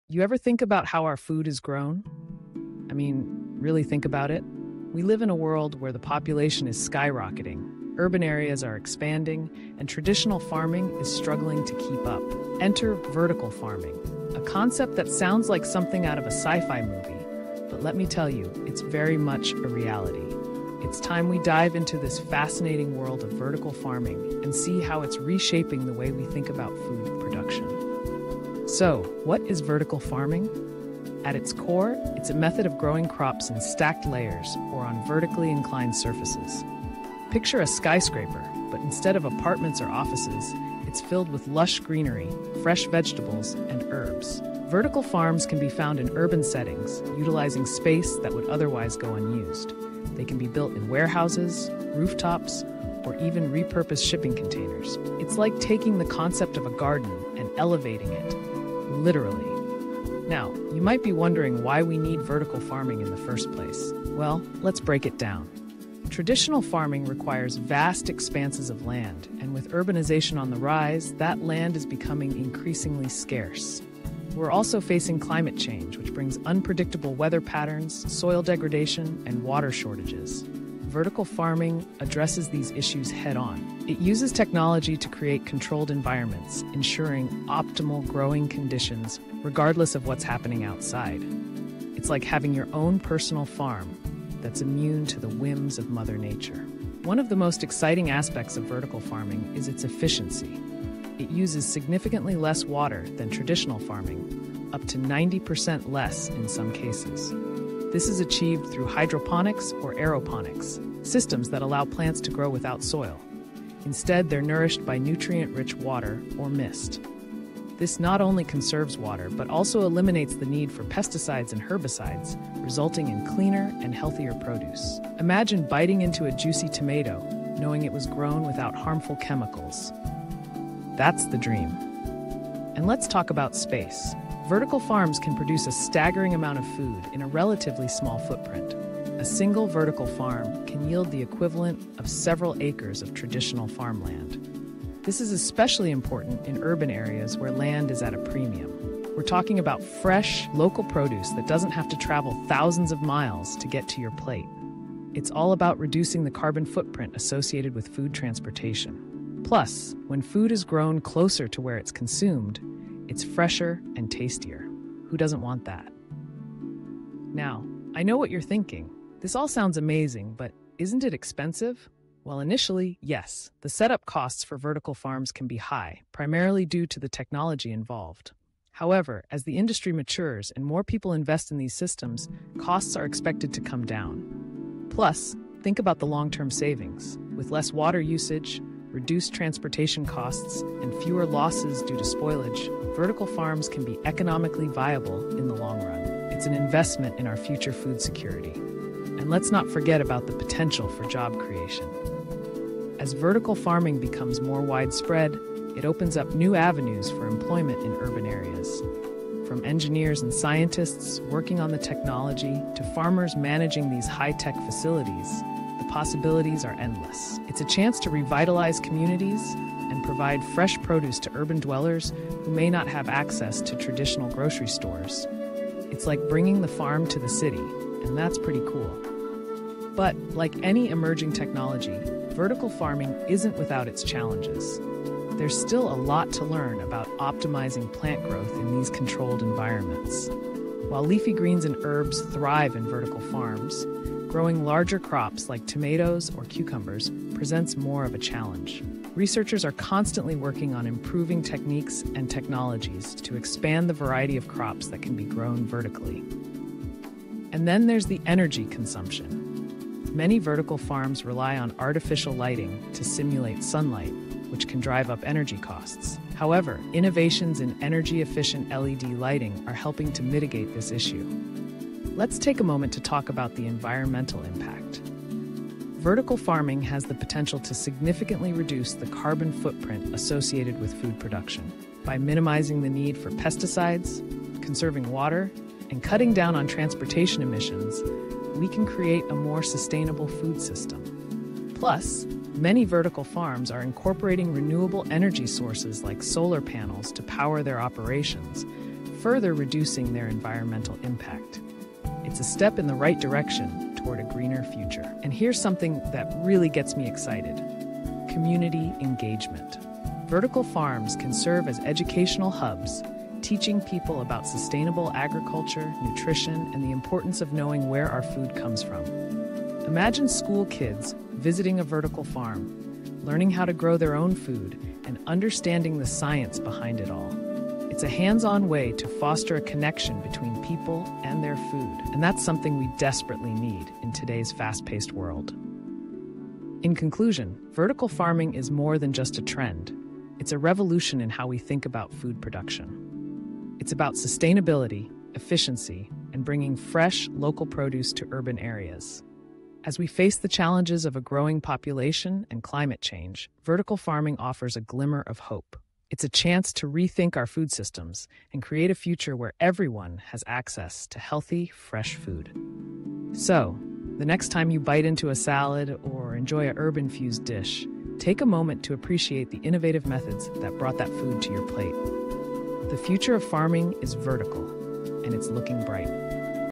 Everything to Know About Vertical Farming| Agricultural Technology
Welcome to our comprehensive guide on verticalfarming. In this video, we’ll explore the innovative techniques, benefits, and sustainability of vertical agriculture. Discover how this method maximizes space, conserves water, and reduces carbon footprints while providing fresh produce year-round. Join us as we delve into the future of FoodProduction.